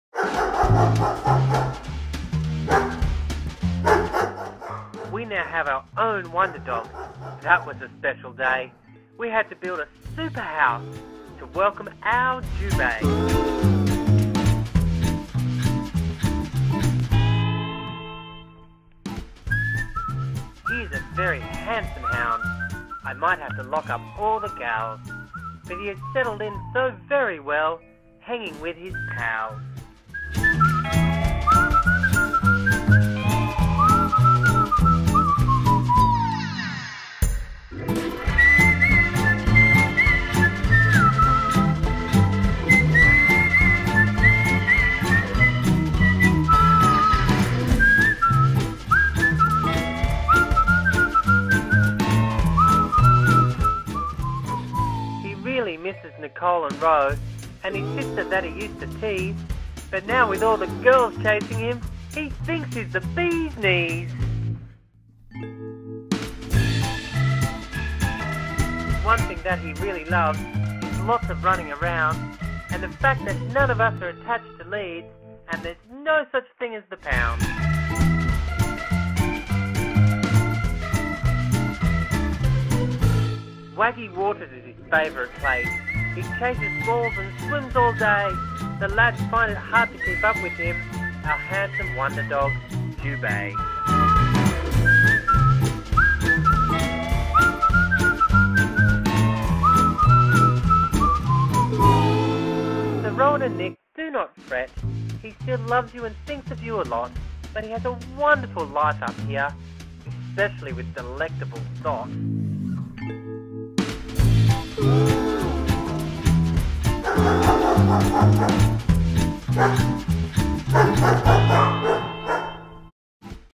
Jubei the Wonder Dog!!
Jubei is a bit of a ladies dog!
Music written and performed by Dominic Halpin.